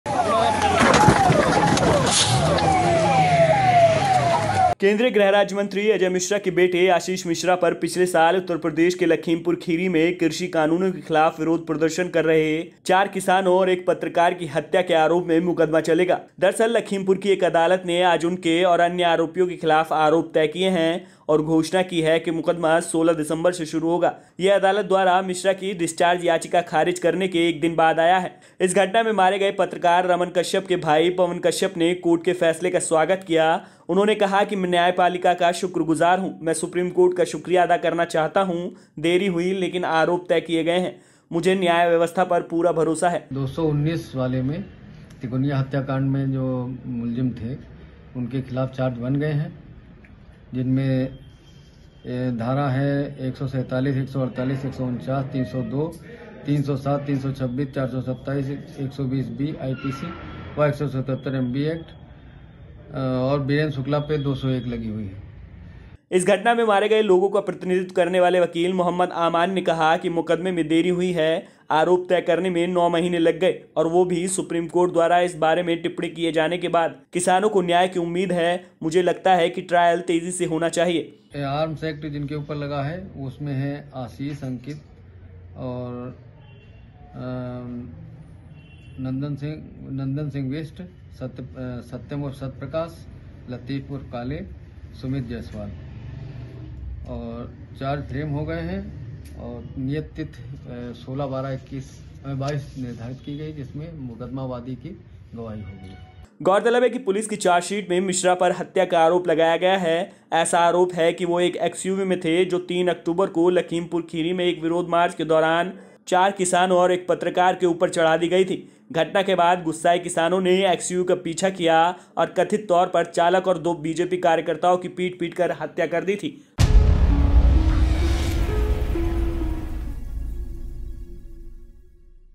Lakhimpur Violence: केंद्रीय मंत्री के बेटे आशीष मिश्रा समेत 14 पर आरोप तय, यह है पूरा मामला|| NEWS
1.Lakhimpur Violence: केंद्रीय मंत्री के बेटे आशीष मिश्रा समेत 14 पर आरोप तय, यह है पूरा मामला|| NEWS
2. केंद्रीय गृह राज्य मंत्री अजय मिश्रा के बेटे आशीष मिश्रा पर पिछले साल उत्तर प्रदेश के लखीमपुर खीरी में कृषि कानूनों के खिलाफ विरोध प्रदर्शन कर रहे चार किसानों और एक पत्रकार की हत्या के आरोप में मुकदमा चलेगा. लखीमपुर की एक अदालत ने आज उनके और अन्य आरोपियों के खिलाफ आरोप तय किए और घोषणा की कि मुकदमा 16 दिसंबर से शुरू होगा. यह अदालत द्वारा मिश्रा की डिस्चार्ज याचिका खारिज करने के एक दिन बाद आया है.